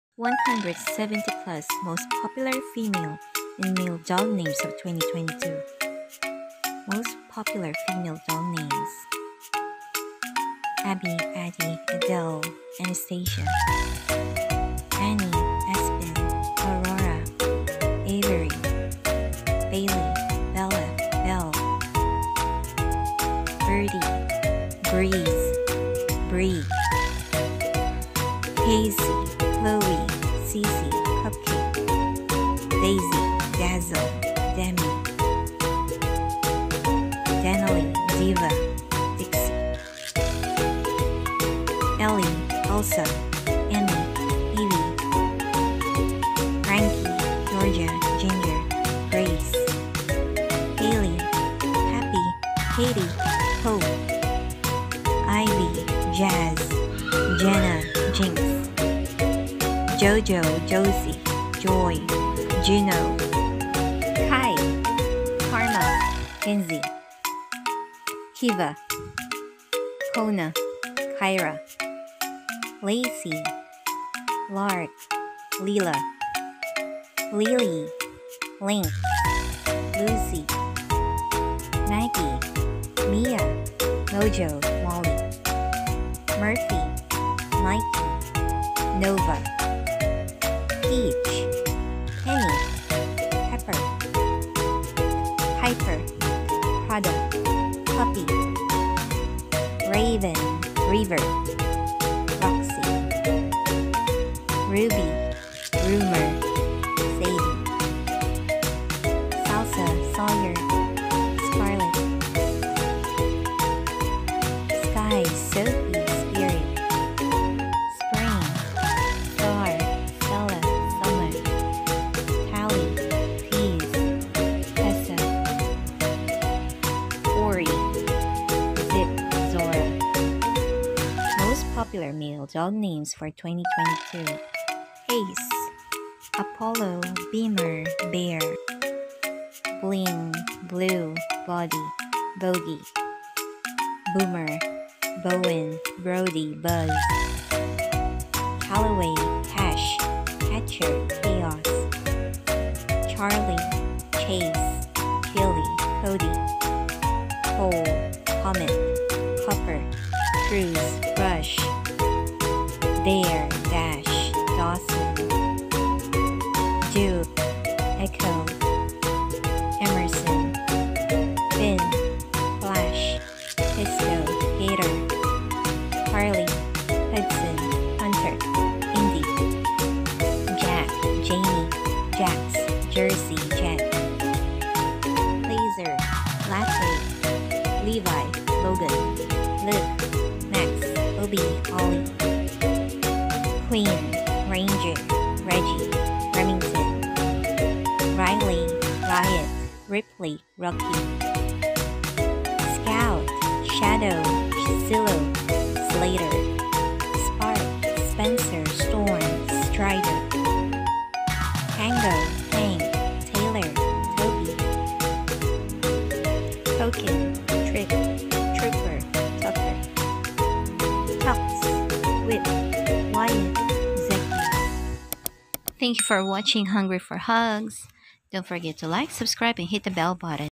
Popular Female and Male Dog Names for 2022| Unique Dog Names for Your Female and Male Dogs
170 Popular Female  and Male Dog Names for 2022| Trending Pet Names for Your Female and Male Dogs

Congratulations on your Fur Baby🐕❤️!!! Here are some of the popular and trending dog names this 2022 courtesy of
Choose a few couple of names which fit your fur baby's personality and sound easy to say. Try which one your baby responds to. Have fun watching!